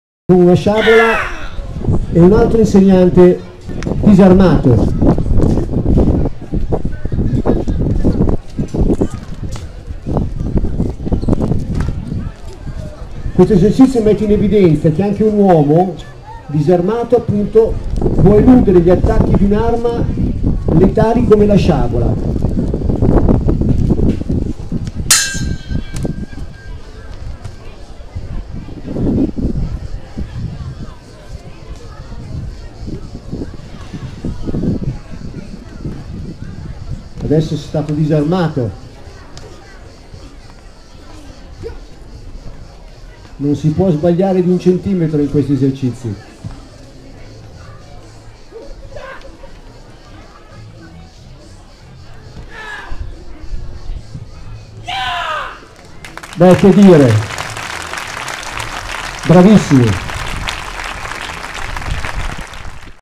Kung Shou T'ui Tan Tao - 功手推单刀 - La mano nuda contro la sciabola - Scuola Chang
功手推单刀
Kung shou t'ui tan tao - 功手推单刀 - La mano nuda contro la sciabola, scuola Chang, maestro Chang Dsu Yao
Eseguito da Kenji Corsano e Riccardo Zappatore

Riprese: Alessandro Borrescio e Alessandro Pinto